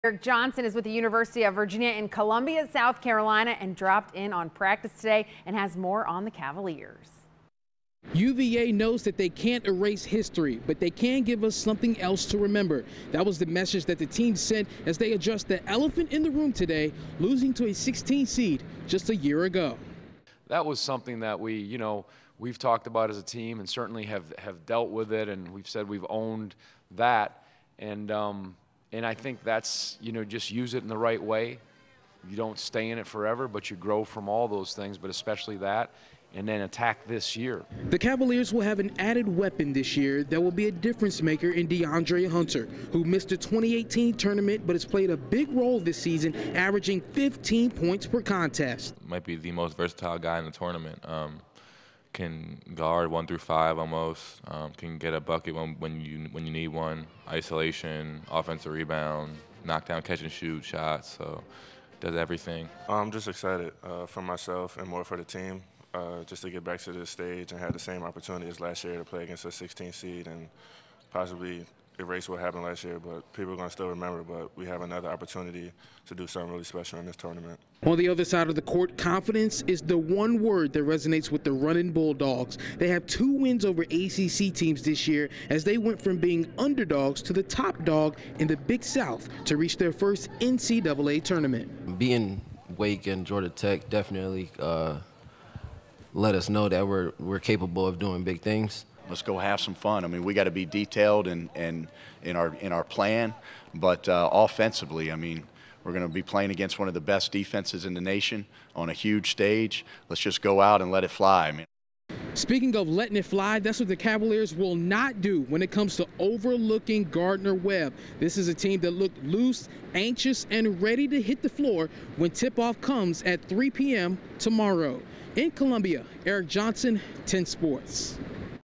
Virginia readies for first round against Gardner-Webb
Virginia knows they can't erase history, but their goal is to give fans something else to remember. That was the teams message as they addressed the elephant in the room, losing to a 16-seed a year ago.